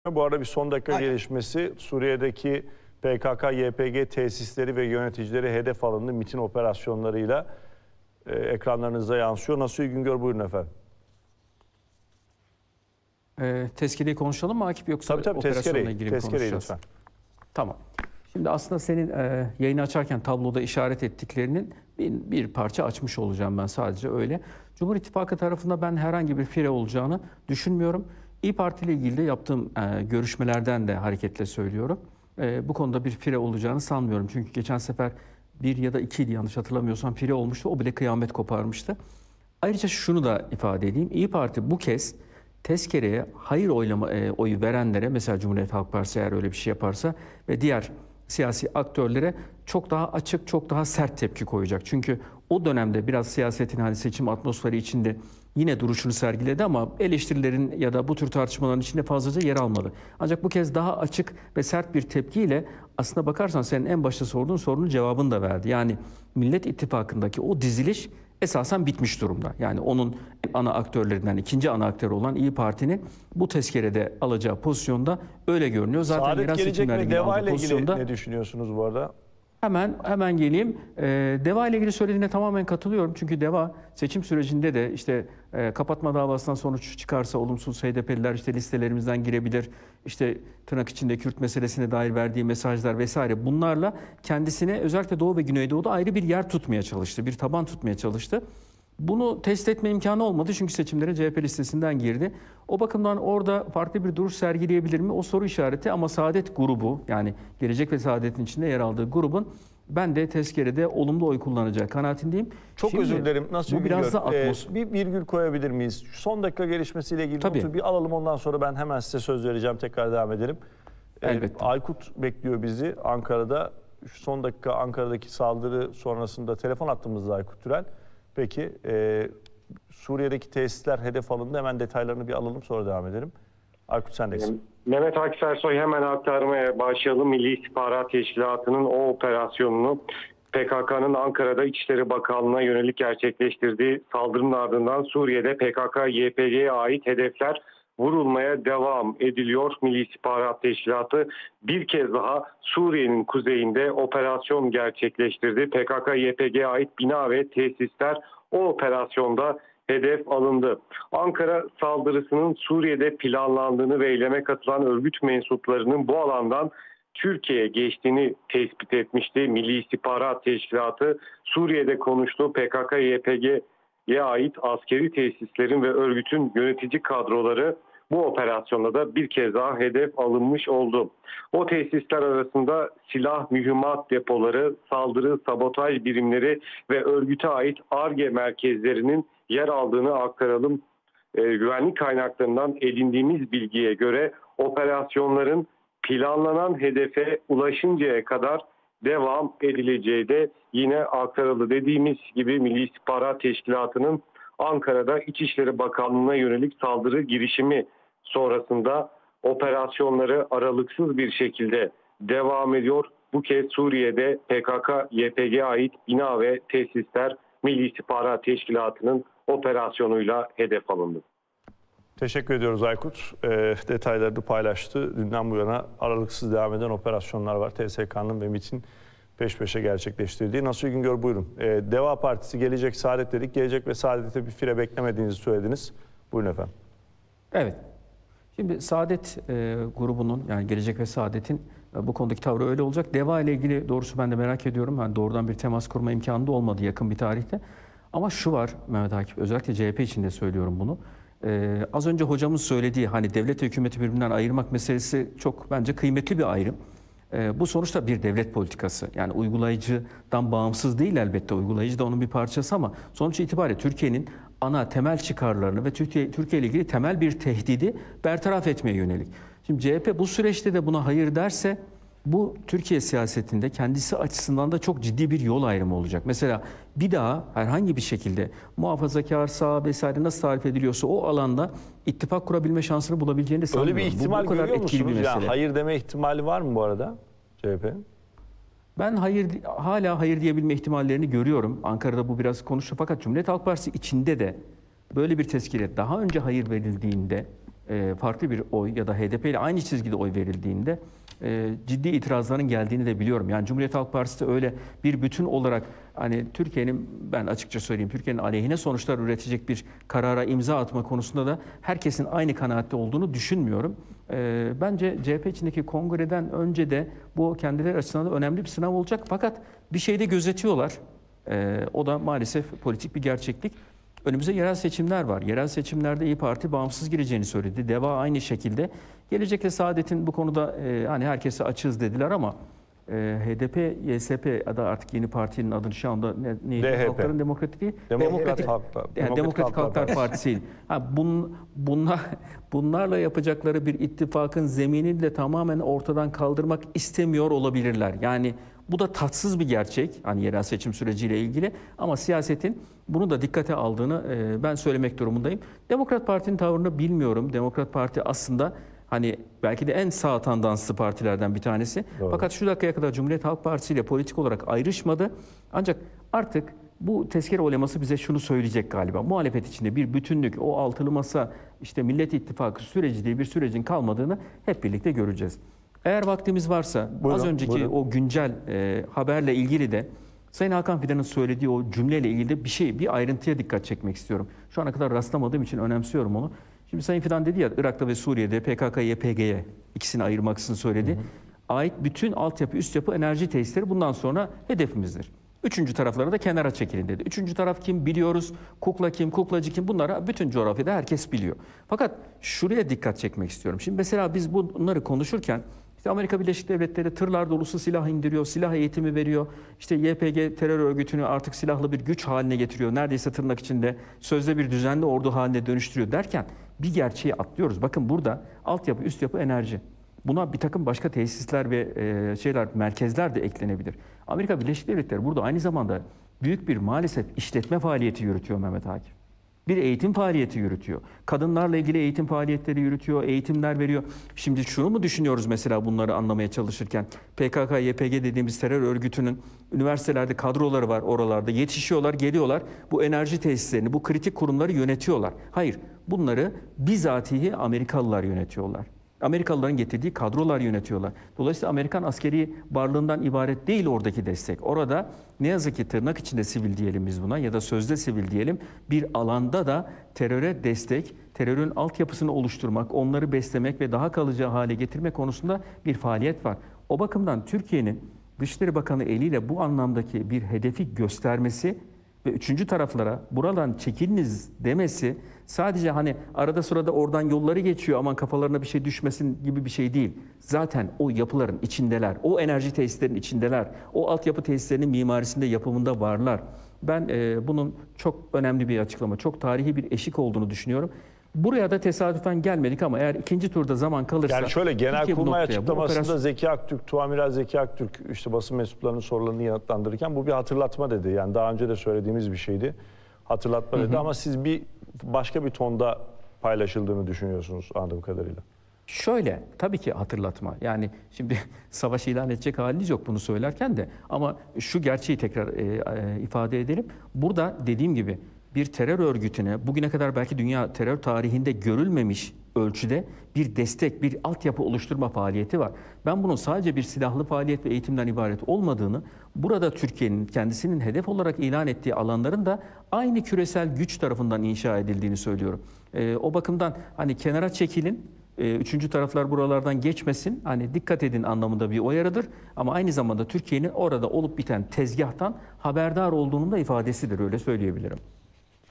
Sınır ötesi tezkeresi TBMM'de. Nasuhi Güngör değerlendirdi
Sınır ötesi tezkeresi TBMM'de... Tezkere oylamasında kim, ne der?

Habertürk Yazarı Nasuhi Güngör değerlendirdi.